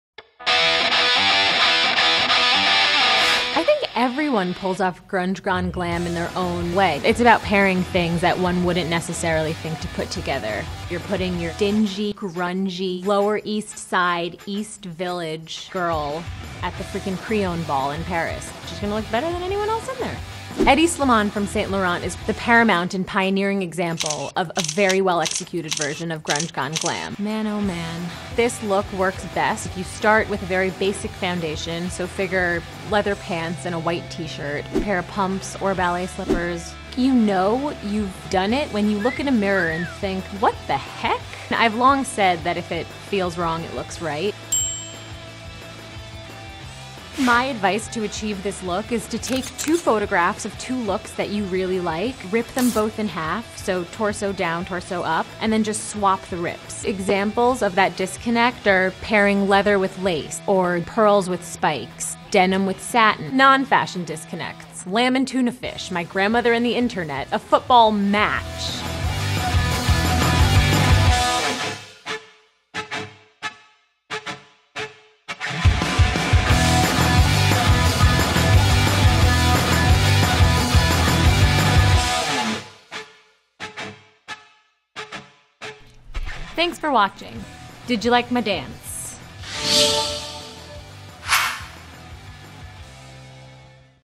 Wearing the Grungy/Glam Look-Fashion Advice w/Man Repeller Leandra Medine-Style.com-In the Mood For
Everyone pulls off the grunge-gone-glam look in their own way. Man Repeller’s biggest piece of advice to pulling off the high-low style? If it feels wrong, it looks right.

Starring: Leandra Medine
Director: Beth Spitalny
Producer: Decoupage Productions

Stylist:  Kathryn Typaldos

Fashion Credits:
Jacket by Saint Laurent
Mesh top by Sandro
Skirt by Araks
Leather pants by Saint Laurent
Choker by Lady Grey
Cuff and bracelet by Eddie Borgo and Giles &amp; Brother
Shoes by Christian Louboutin
White button-down by Alex Mill
Vintage jeans from Where I Was From